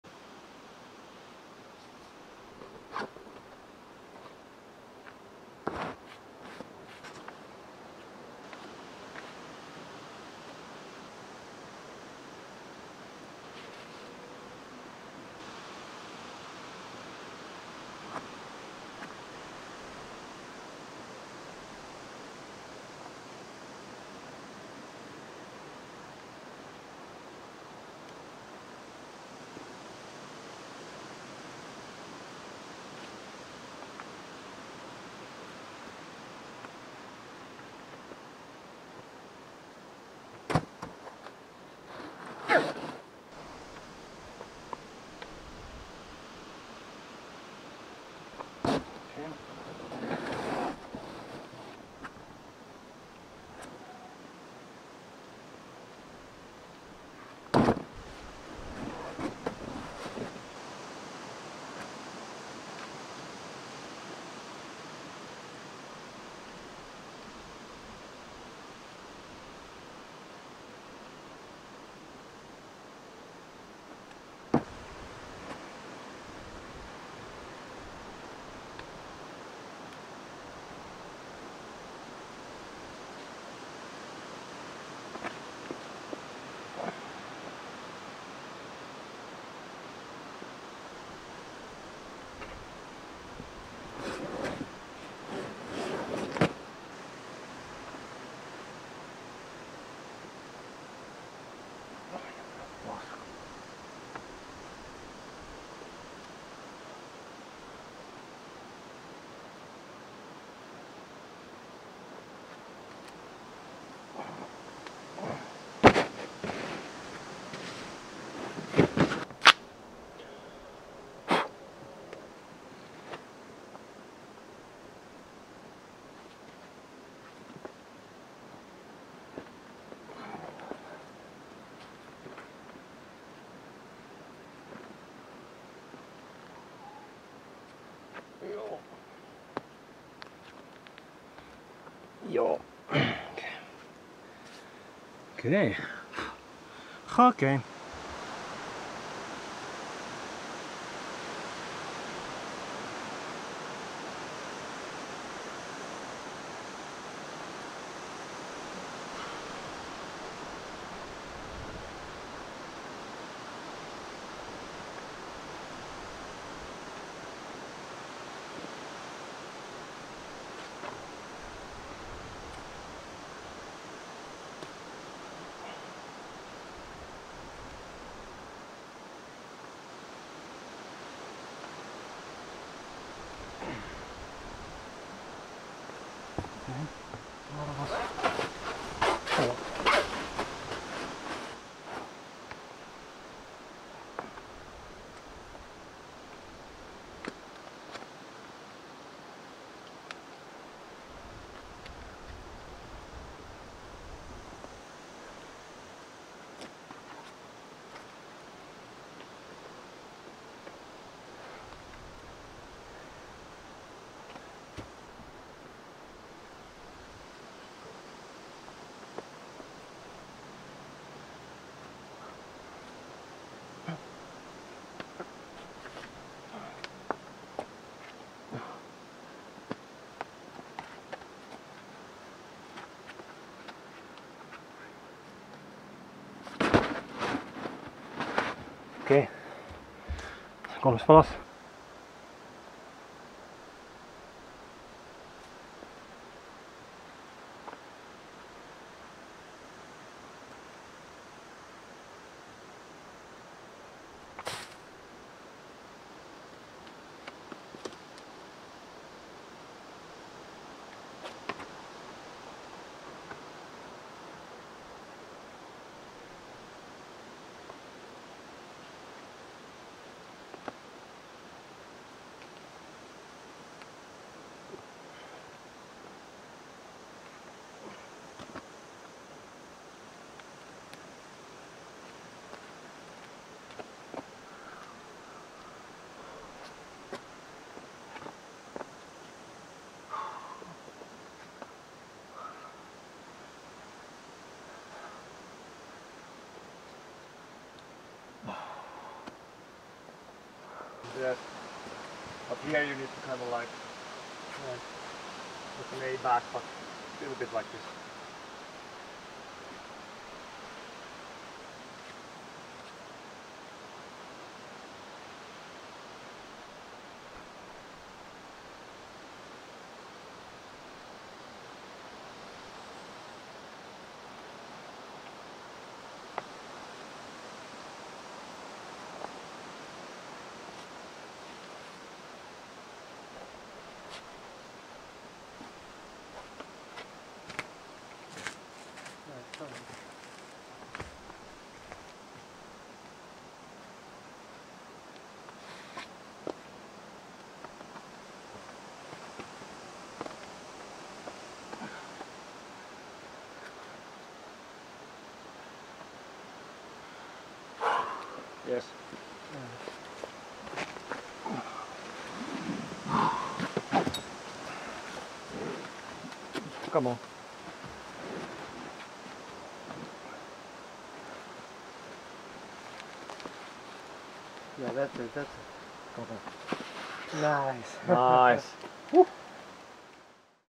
Pornainen Climbing vlog 2022-08
0:00 Working the moves to Spiritual 7A
5:00 Link-up